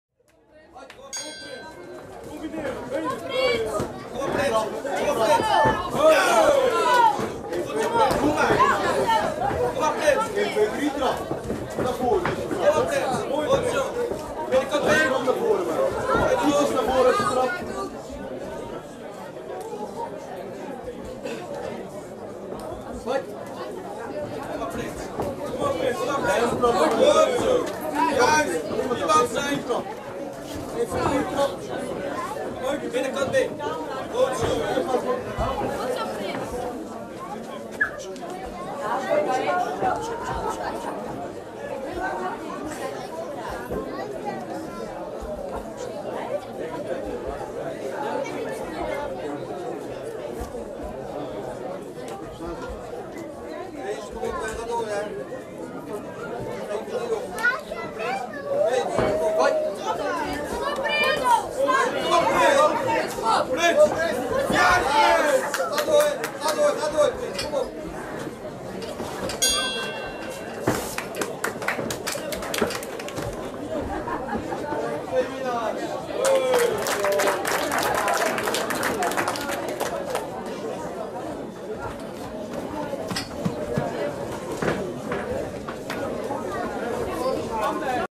KICKS - Prince Minnema de la Cruz vs Mazen Ahmed
Jeugd partij 2x1 minuut (DEMO)
Kicks Ridderkerk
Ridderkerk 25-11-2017